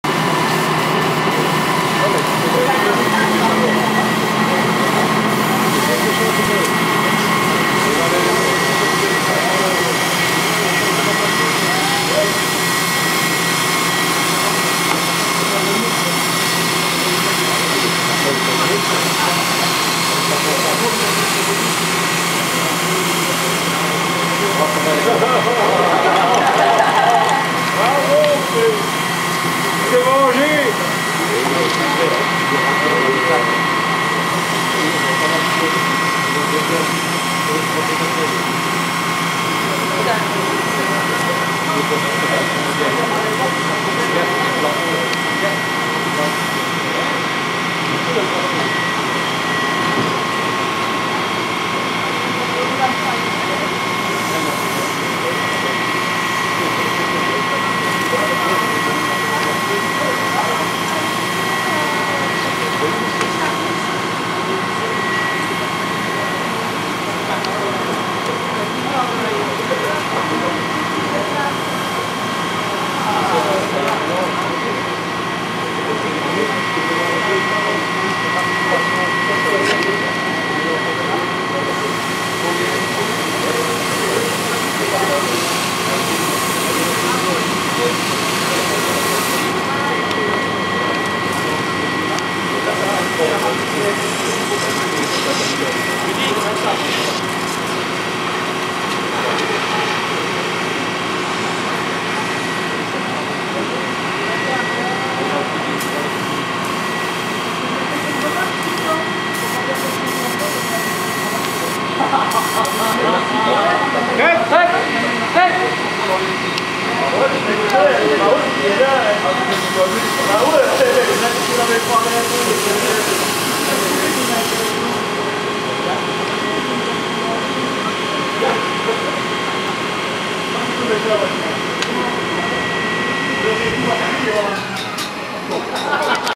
Journée S.O.S. weekend aux Pompiers de Marche-En-Famenne
démonstration d'extinction de véhicule.